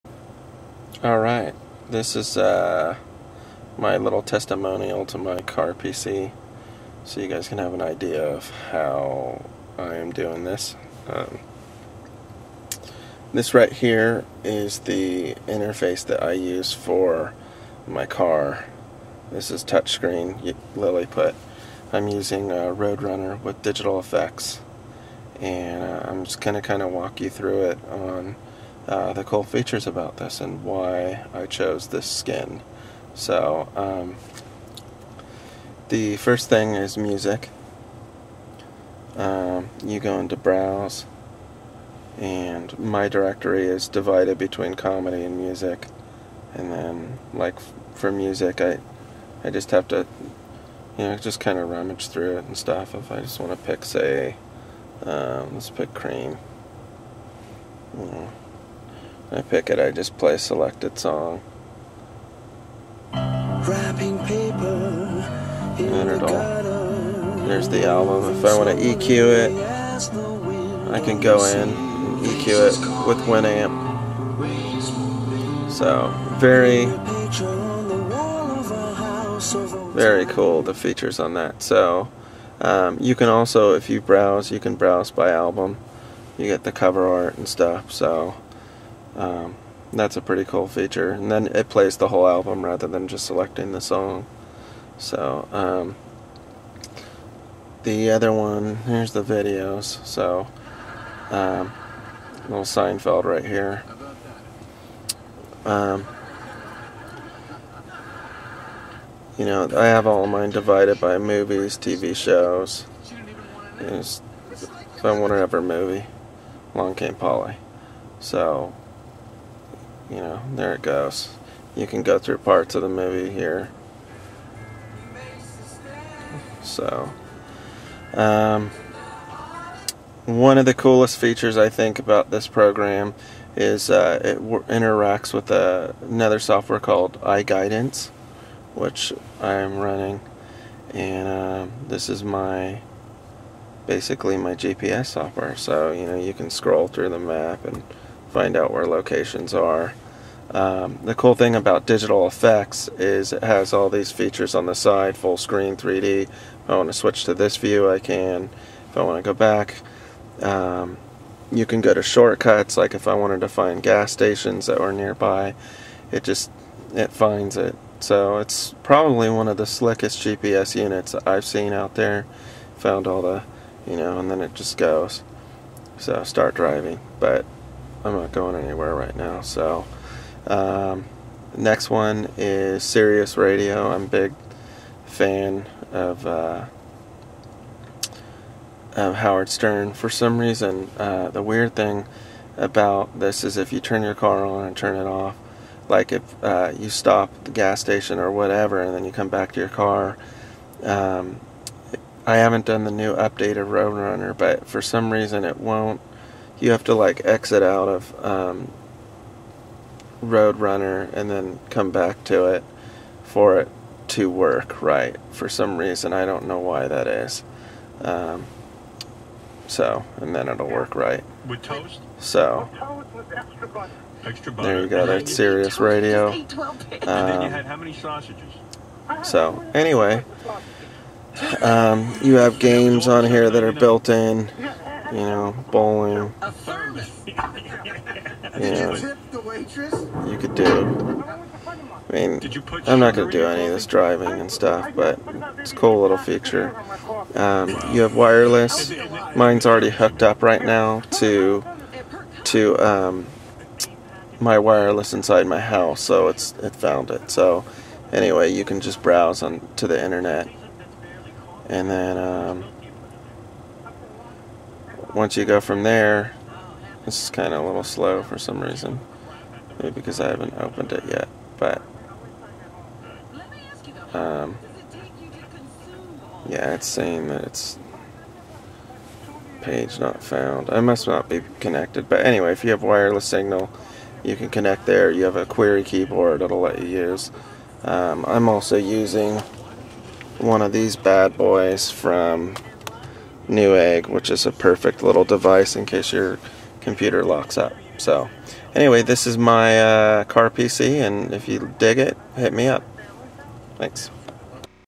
My CarPC with Roadrunner & Digitalfx
CarPC with Roadrunner & Digitalfx 4

You can download Road Runner for free.  It now called Ride Runner.

The software is kinda buggy. The skin I am using is Digiital FX.